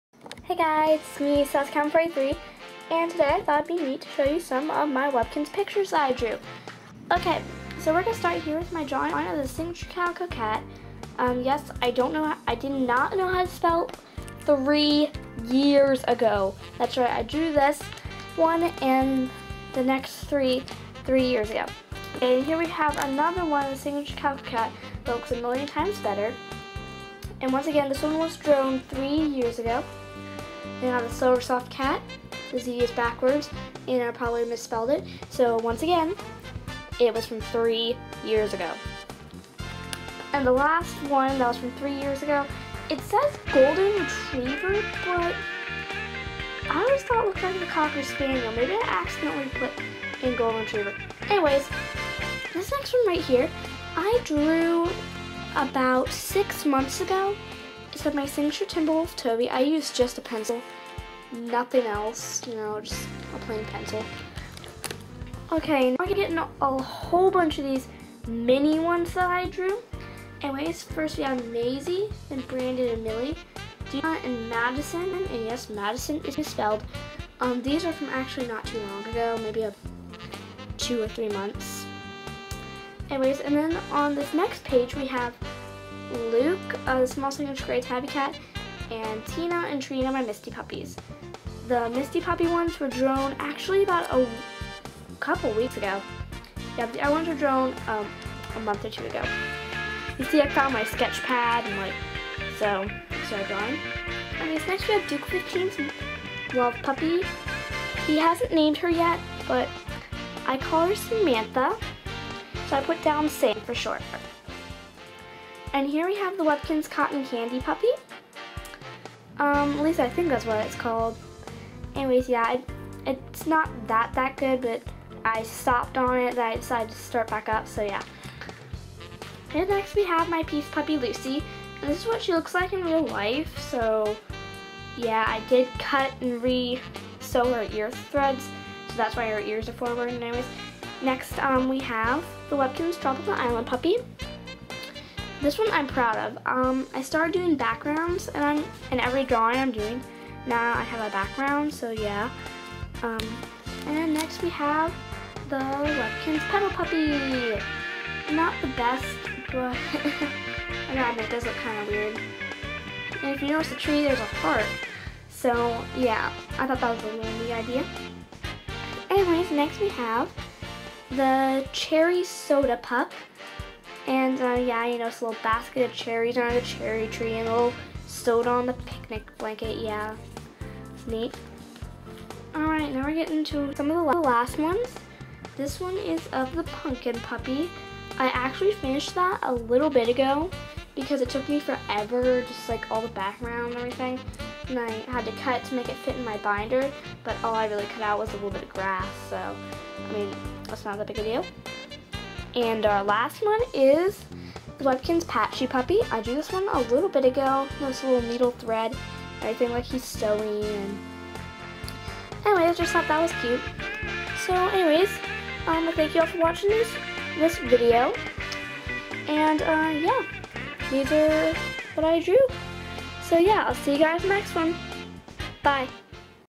My Webkinz Drawings
Sorry the pictures dont look that good. The scaner whited them out and i had to turn the brightnees down. :(